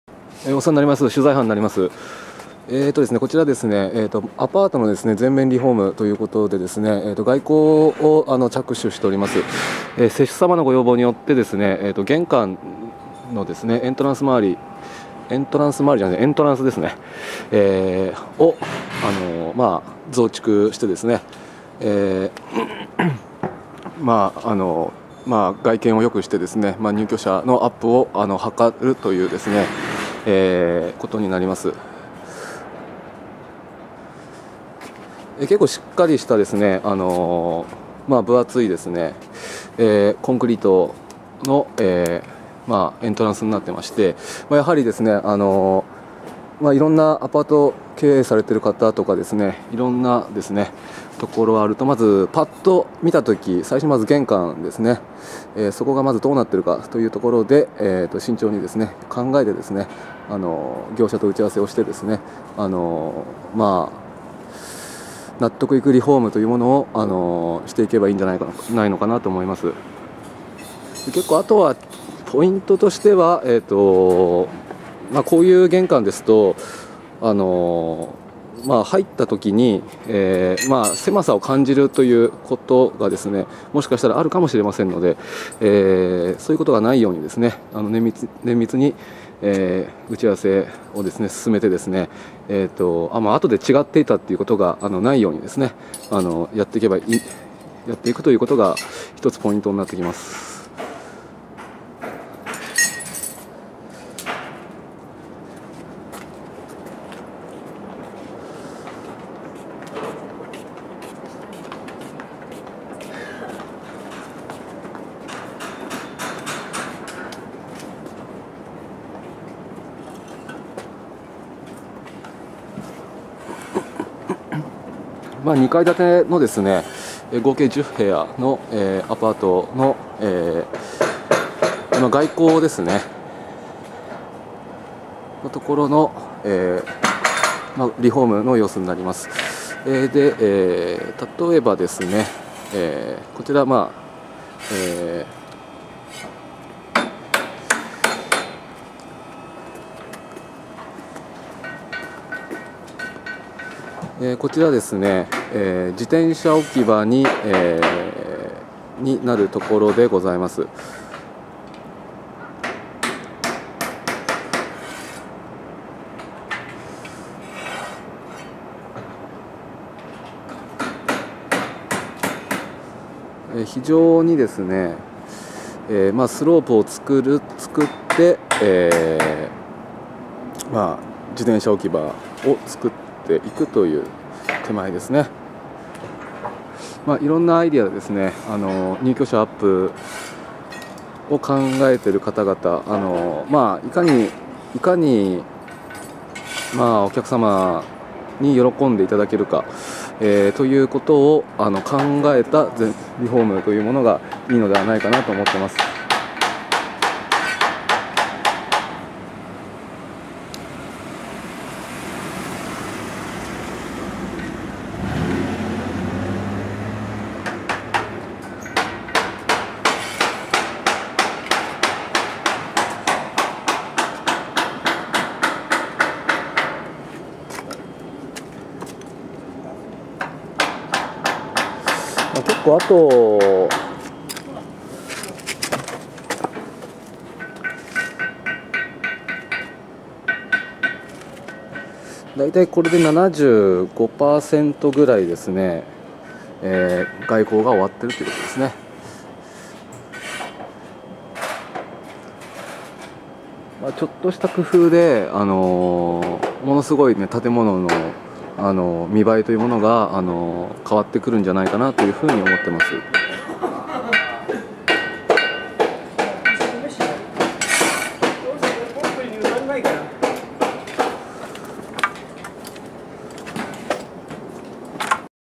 アパート・マンション経営での工事でのエントランス（玄関）
リフォームプロ取材班 のWEBをよかったらご覧くださいませ。

マンション経営/アパート経営における古くなったアパートやマンションも外溝を増築することにより、オートロック形式にしたり・・・見栄えを変えたりと・・・特に消費者がきにする玄関や見栄えという点についてです。

さらに、空いている場所があればそれを有効活用して自転車置き場などにするという空間を活かした施工事例の取材になります。